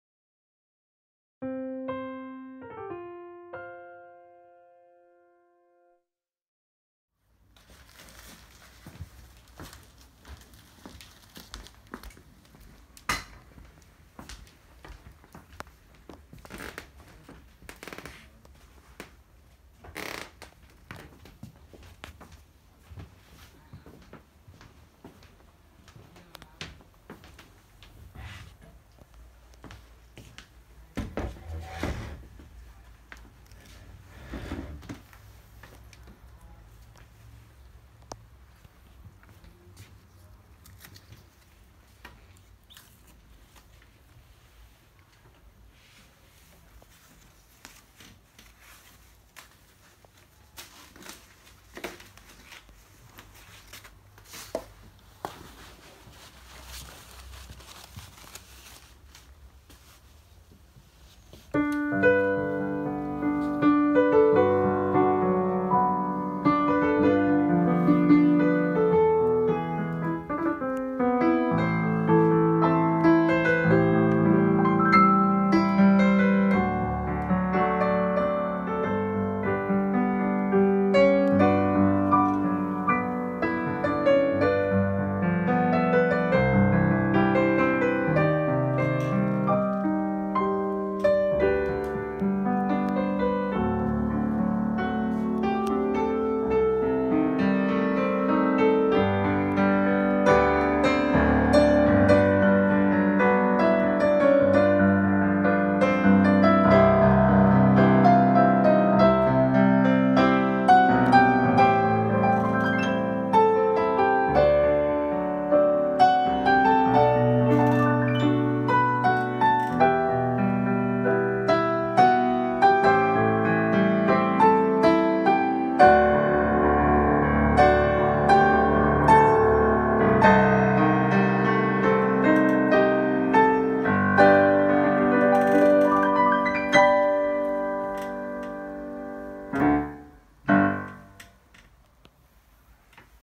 Yamaha CLP-695GP First Look | Digitalpiano.com
Vi har lige fået Yamaha CLP-695GP til butikken i København, og tager et hurtigt blik på hvordan det lyder og spiller. Se mere på digitalpiano.dk - Danmarks største udvalg!